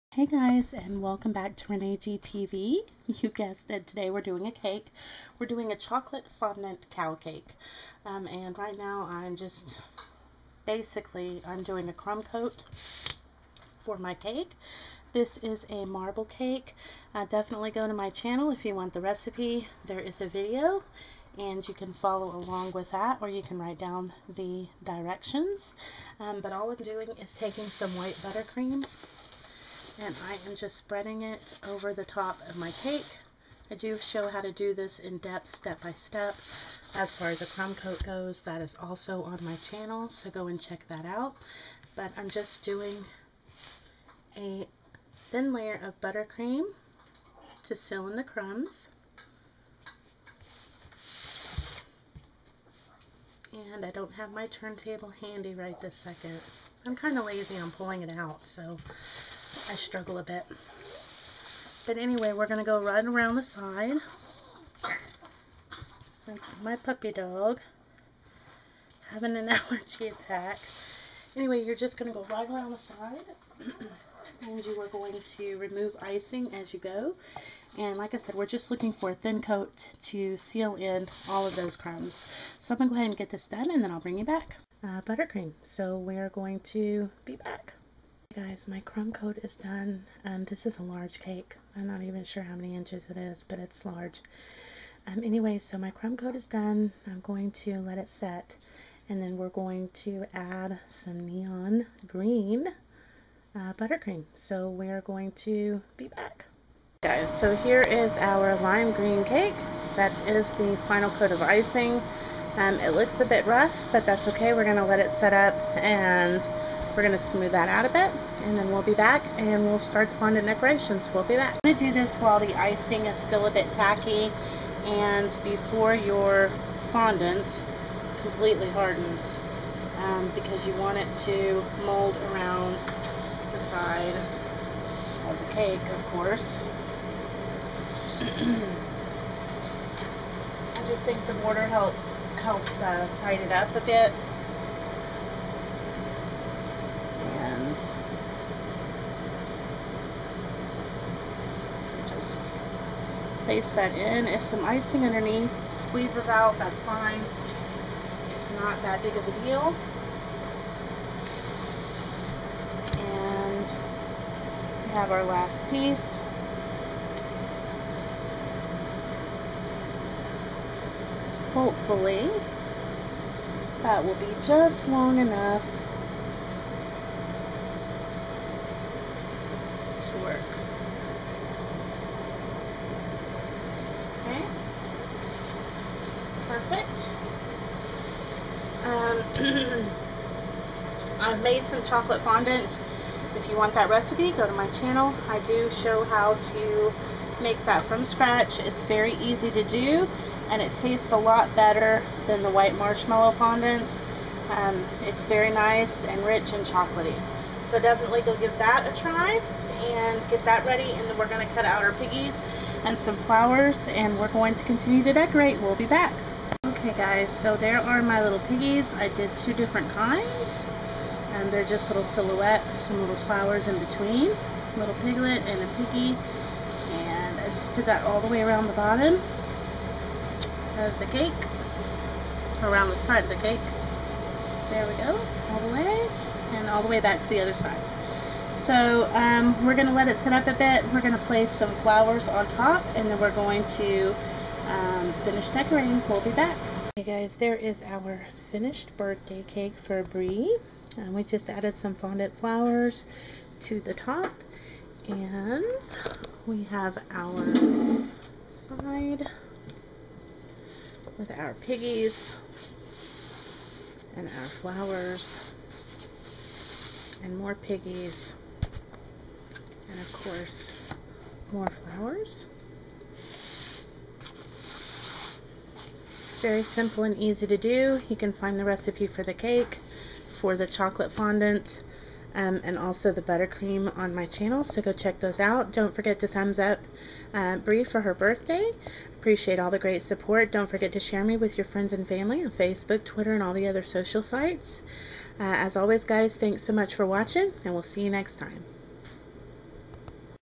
Chocolate Cow Birthday Cake
Easy to make chocolate fondant cow decoration for a birthday cake.